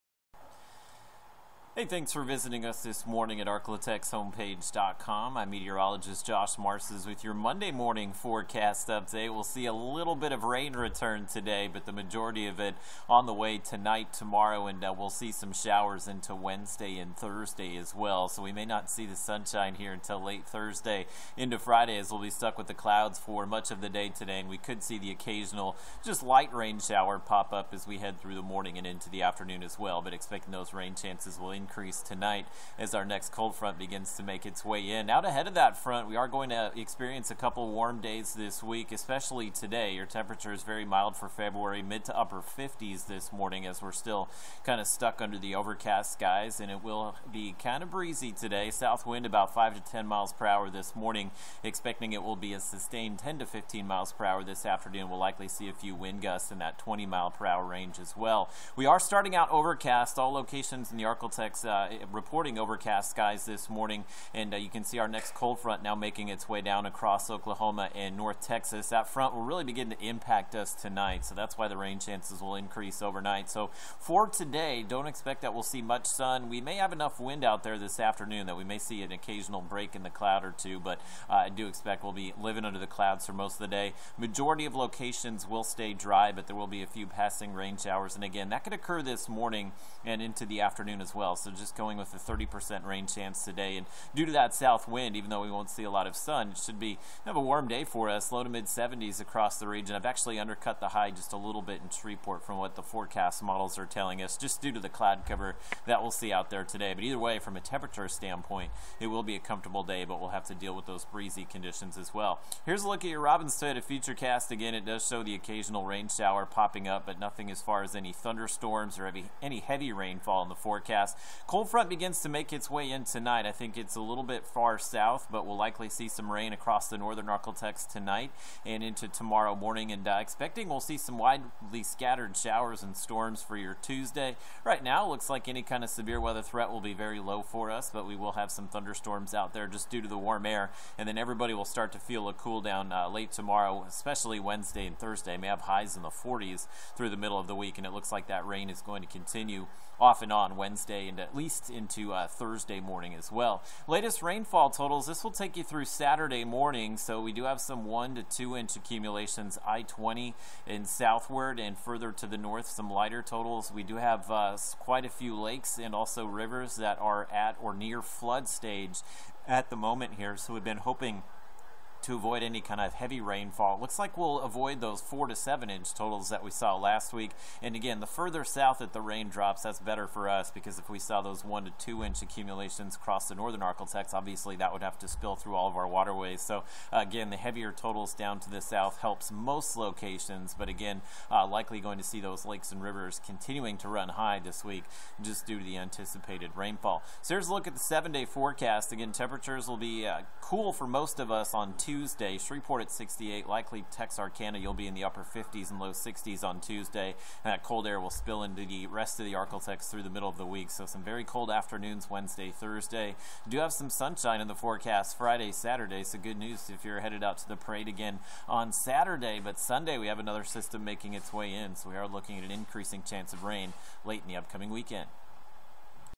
Arklatex Weather Outlook Mon 2/17 a.m.
Weather webcast containing the latest seven day forecast for Shreveport, Texarkana, and all of the Arklatex.